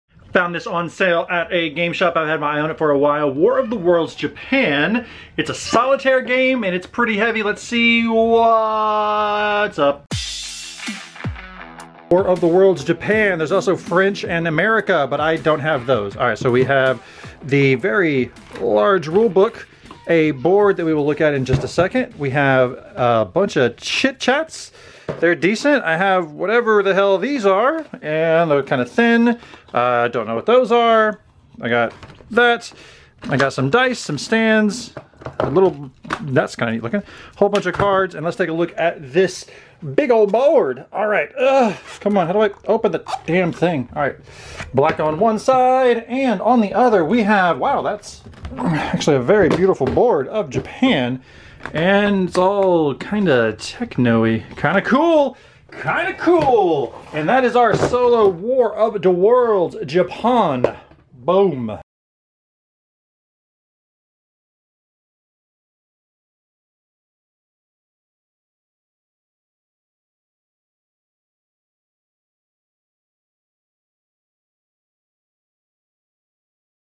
Unboxing The War of the Worlds: Japan (because why the hell not)
Everyone else has unboxing videos, so why not me?

But I know you got shit to do, so I’m not gonna take all day to show you what you wanna see.

Get your cameras ready folks, this ain’t gonna last long